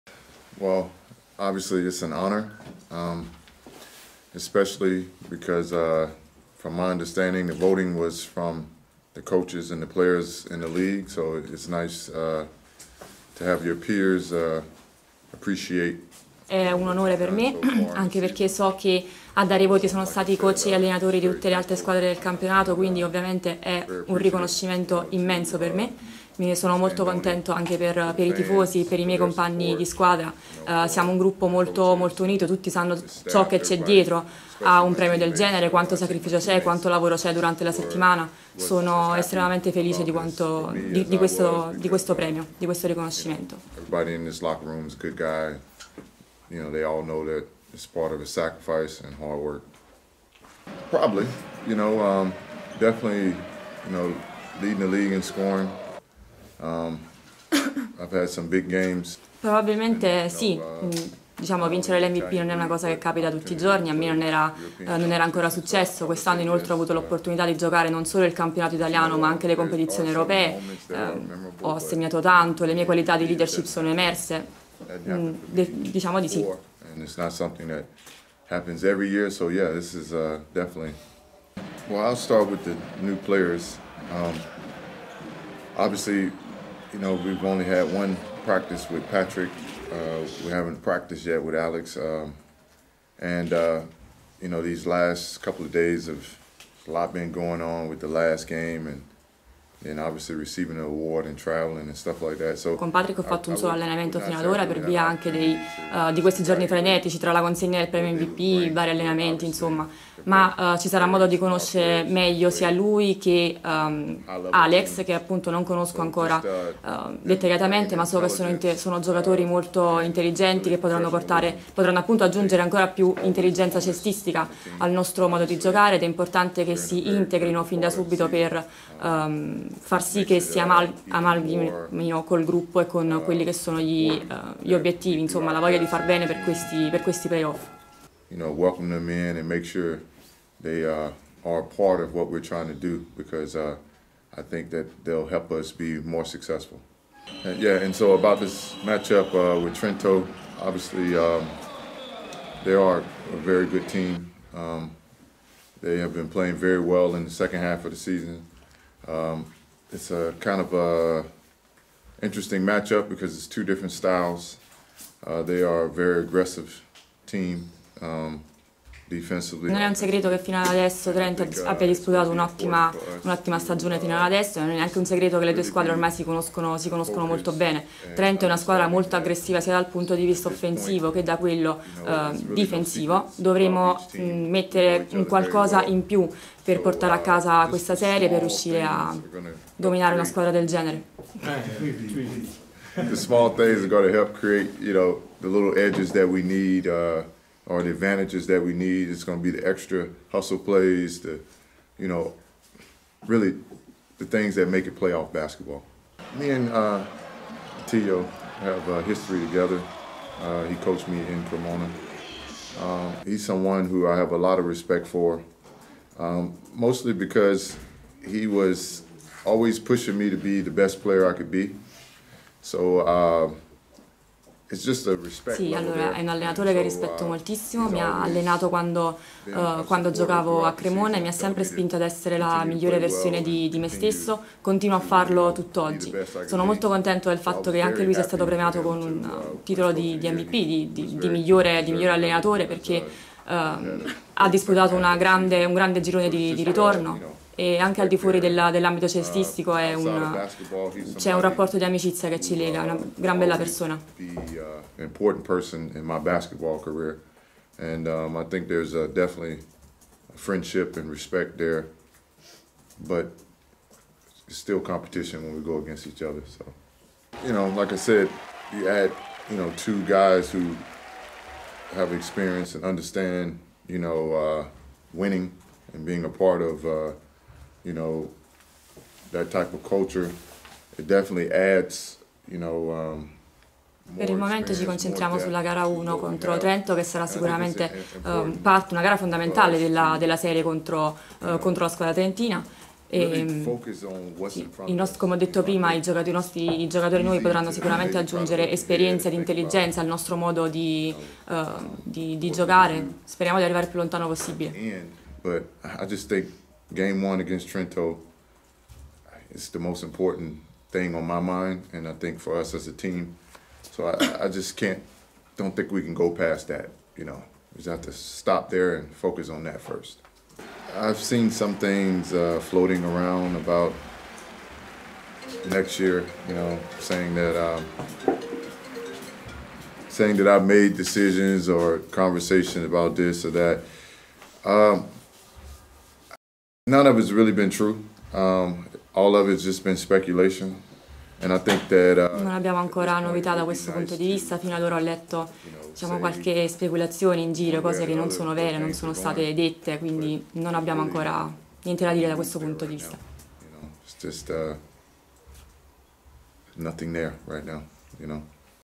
Rich: "Sotto con Trento, nessuna novità sul mio futuro"
La guardia della Sidigas Avellino è felice per il premio di MVP del campionato.